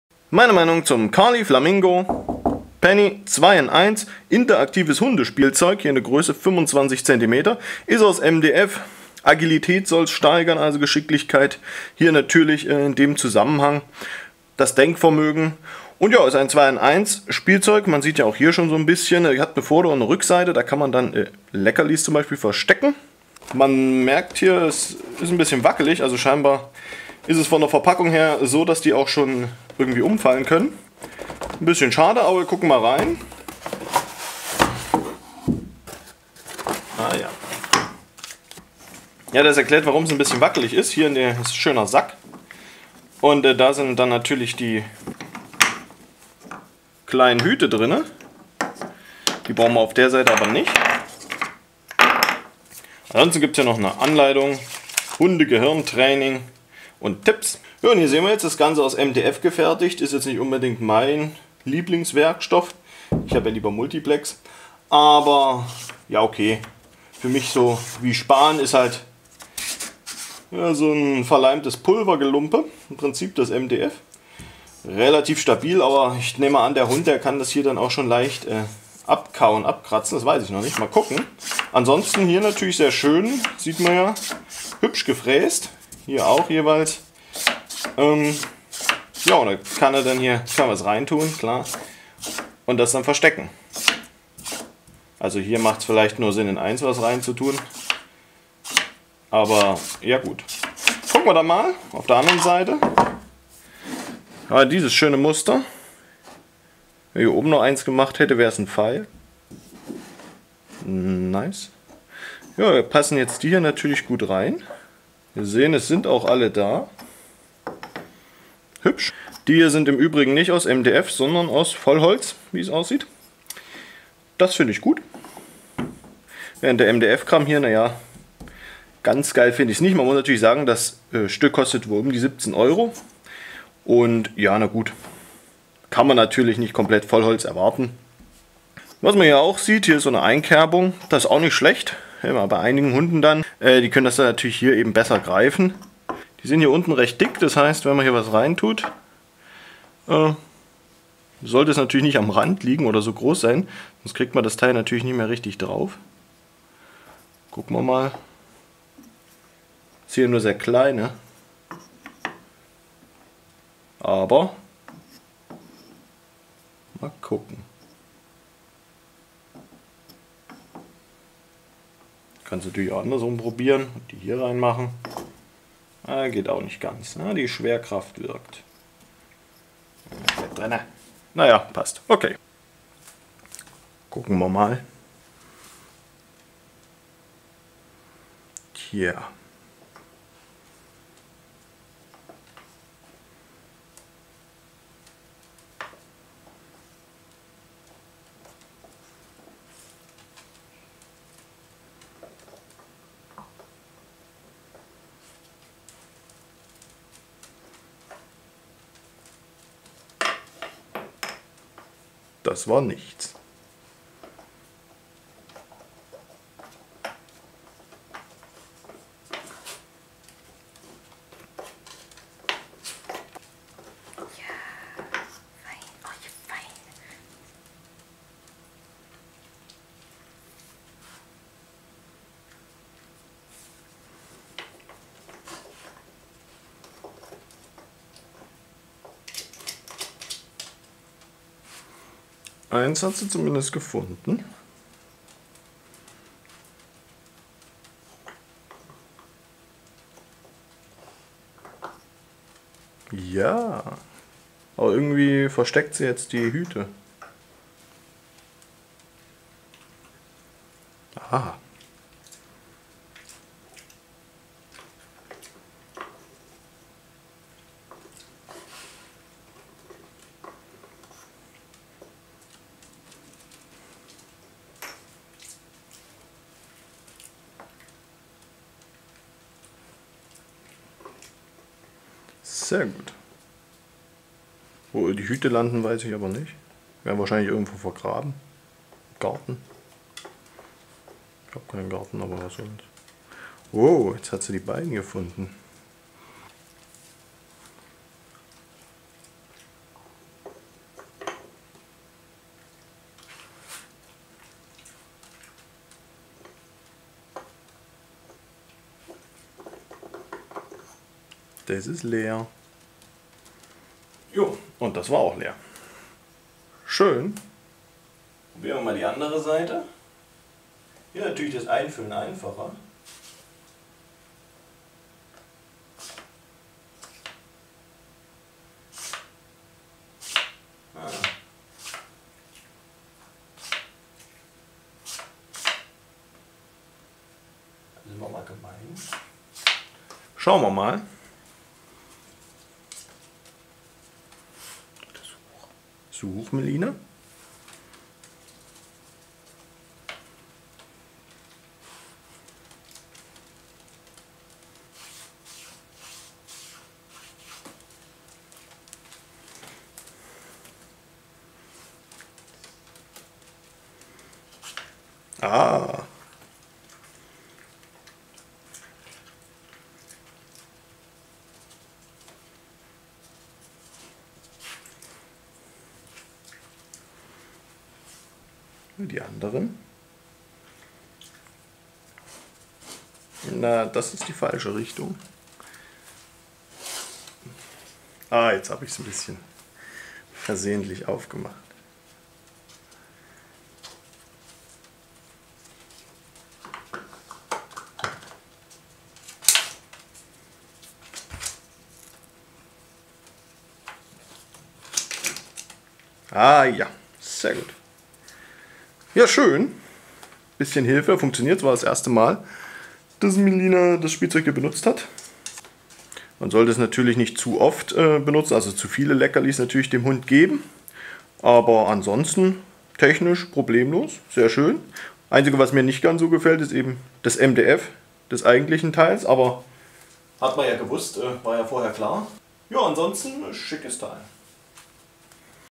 Karlie Flamingo Penny - 2 in 1 Hundespielzeug Hund Training | Unboxing, Aufbau, Test und Review
Für Video-Navigation "MEHR ANZEIGEN" anklicken.
Meine Meinung zum Karlie Flamingo Penny 2 in 1 Hundespielzeug.
Unboxing, Aufbau beider Seiten, Test mit Hund und Leckerlies

Video-Navigation:
 - 00:00 Unboxing
 - 00:58 Allgemeines und erste Seite mit Slidern
 - 01:42 Zweite Seite mit Hütchen
 - 02:26 Ein paar Leckerlies rein
 - 03:03 Test der Seite mit den Hütchen
 - 05:18 Test der Seite mit den Slidern
 - 06:49 Kurzes Fazit